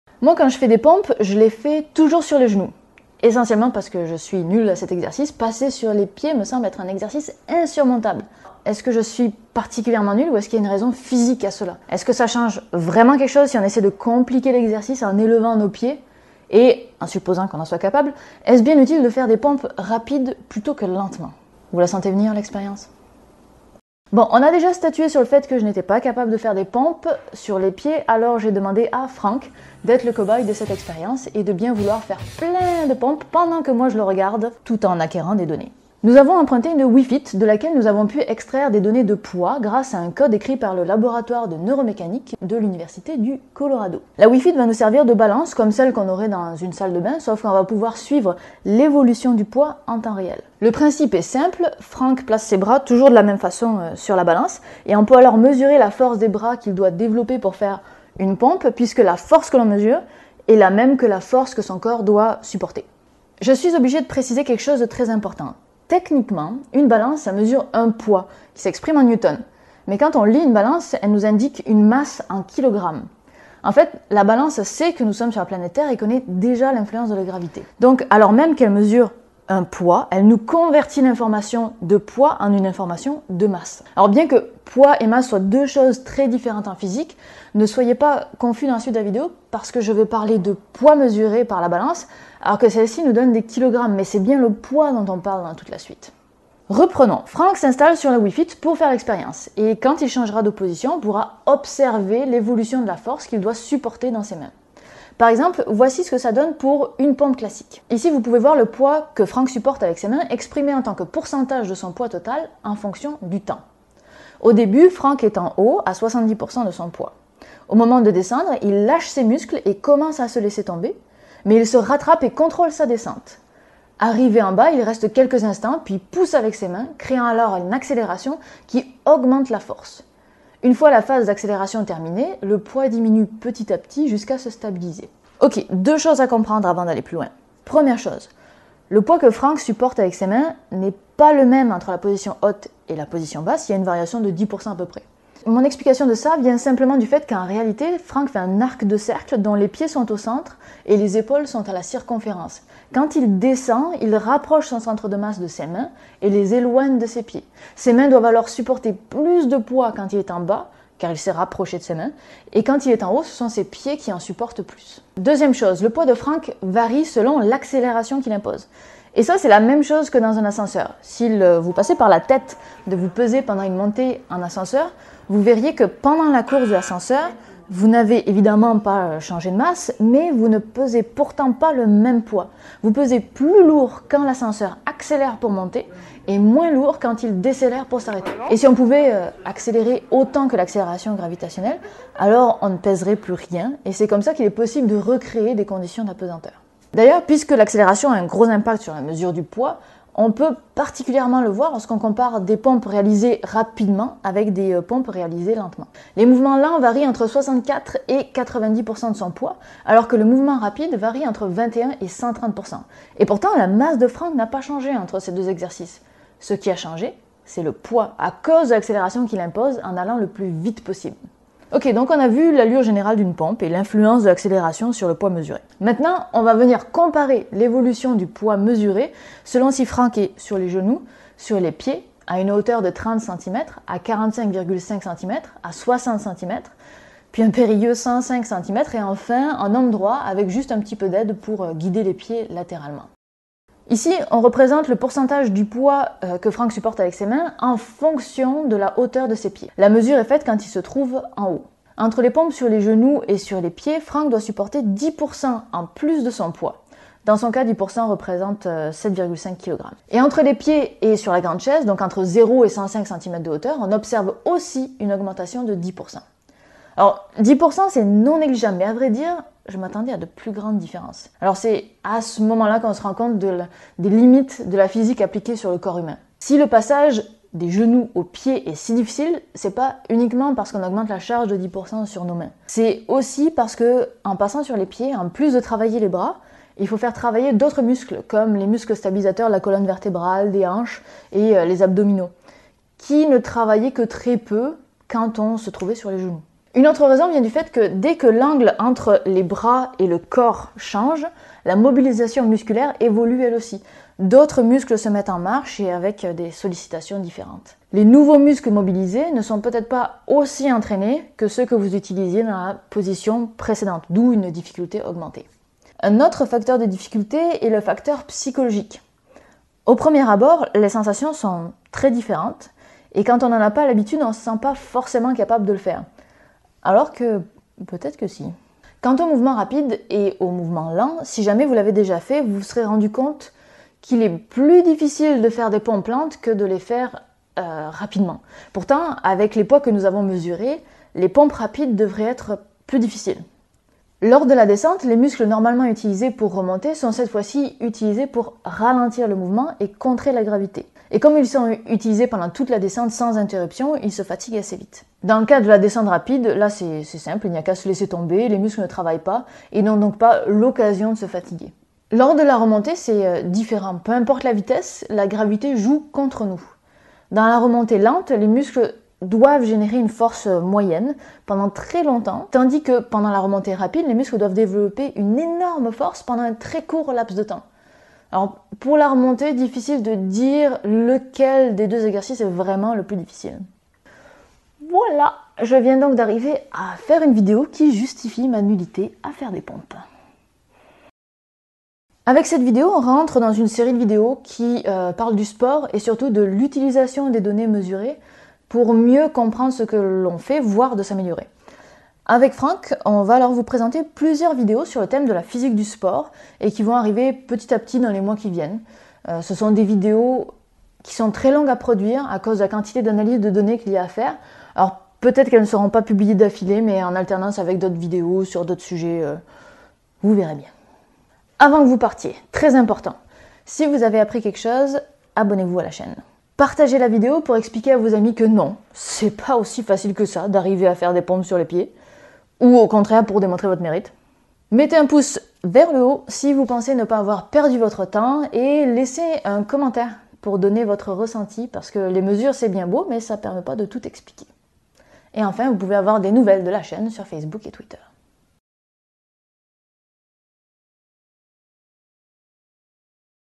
Pourquoi les pompes en hauteur sont-elles si difficiles ?
Les pompes (push-up en anglais) sont un des exercices classique que l’on réalise lorsque l’on fait du sport. Mais dès que l’on change la hauteur des pieds, la difficulté change. Mais pourquoi change-t-elle et à quel point ? Expérience !

Note qui vous fera gagner du temps : il faut absolument un matlab 32bits pour le faire fonctionner

**Contreras, 2012, The Biomechanics of the Push-up: Implications for Resistance Training Programs

**Anderson, 2013, Comparison of EMG activity during stable and unstable push-up protocols

**Cogley, 2005, COMPARISON OF MUSCLE ACTIVATION USING
VARIOUS HAND POSITIONS DURING THE PUSH-UP EXERCISE

**Eben, 2011, KINETIC ANALYSIS OF SEVERAL VARIATIONS OF
PUSH-UPS

**Freeman, 2006, Quantifying Muscle Patterns and Spine Load
during Various Forms of the Push-Up

**Bigland, 1954, THE RELATION BETWEEN FORCE, VELOCITY AND
INTEGRATED ELECTRICAL ACTIVITY IN HUMAN MUSCLES

**Baumgartner, 2009, Objectivity, Reliability, and Validity for a Revised Push-Up Test Protocol

**Chou, 2010, effect of push up speed on elbow joint loading

Remerciements :
Planification expé/expérimentateur/relecteur : Franck Le Naveaux
Aide à l’interprétation des données : Kate Rousseau Harrison
Adaptation du script informatique/Caméra : Manuel Vonthron
Expérience de l'ascenseur : Laure Boyer
Prêt de la Wii fit : Neige Denise Girouard et Patricia Girouard Nobl

Et merci à tous les tipeurs !